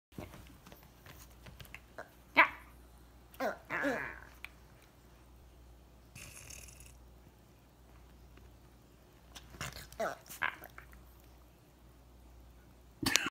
Mom and Rosie Snarling in Havertown
Lisa allows Rosie to believe she can win a fight for dominance!